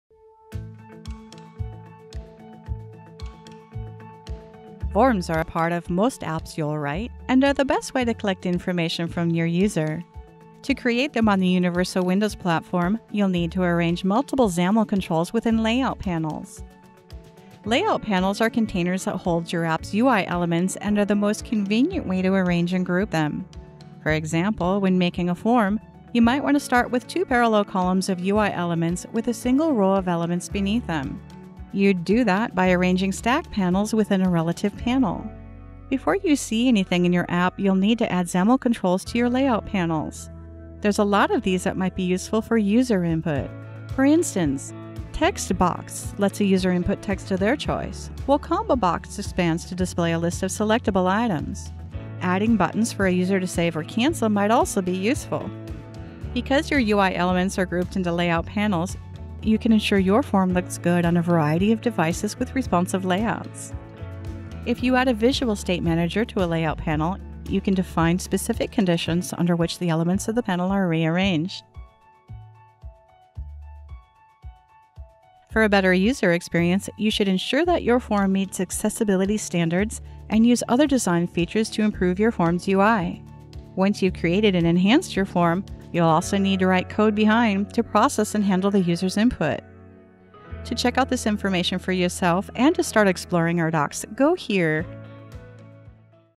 Get Started for Devs: Create and customize a form on Windows 10
Our Get Started docs for Windows developers have been updated to give you hands-on experience with basic tasks in app development on Windows 10. This new video takes you through one of those topics, and covers the basics of creating a form in your app.  To find more of these docs, head